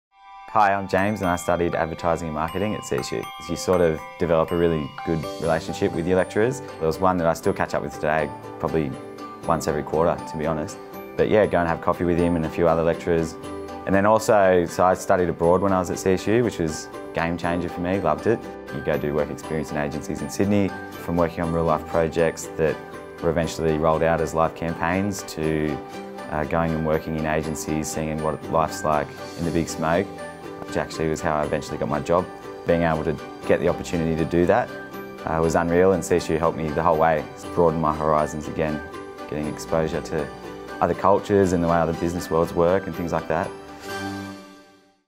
START NOW: James has a bright future thanks to CSU.
James developed strong relationships with his lecturers and enjoyed seeing his advertising projects rolled out into live campaigns. Through work placement, James was able to secure full-time employment after graduation.